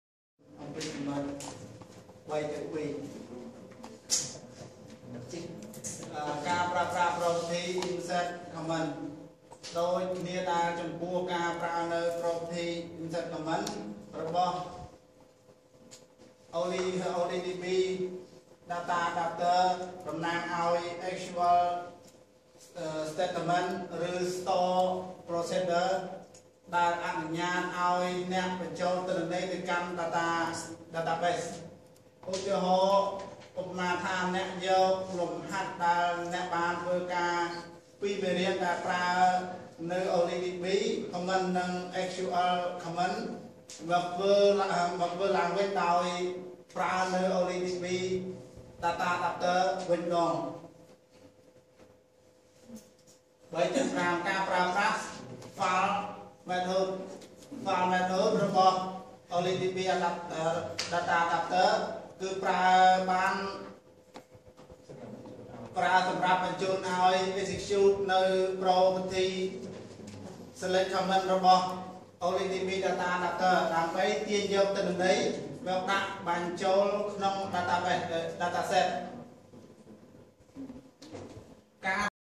2-Advanced C#: Chapter 5 DataAdapter, DataSet and DataTable; Group E; Presentation by Mr. Khom Dara
Advanced C#: Chapter 5
DataAdapter, DataSet and DataTable

This video demonstrate how to using Insert Command Property.

Group E
Presentation by Mr. Khom Dara, Part 2
Batch 18, Evening Class (A), Bachelor Degree
Faculty of Science and Technology
Asia Euro University